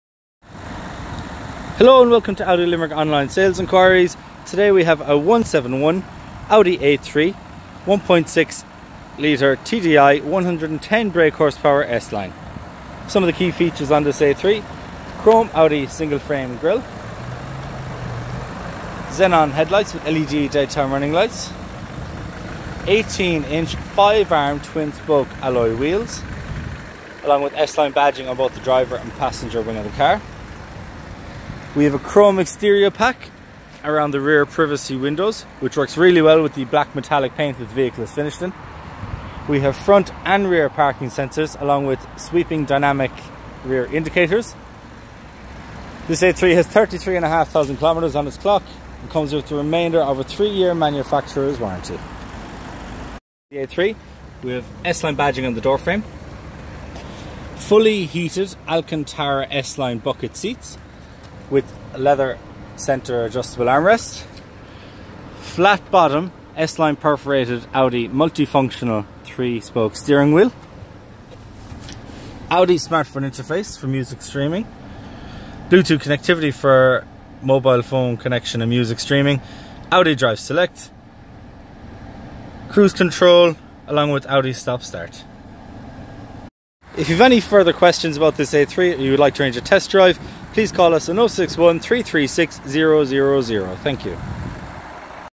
171D23645 - 2017 Audi A3 1.6TDI 110 S-LINE SALOON FROM ONLY 259 PER MONTH 2...
Here is a video of our 171D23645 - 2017 Audi A3 1.6TDI 110 S-LINE SALOON FROM ONLY 259 PER MONTH 28,495. Please contact Sales to arrange a test drive today. Audi Limerick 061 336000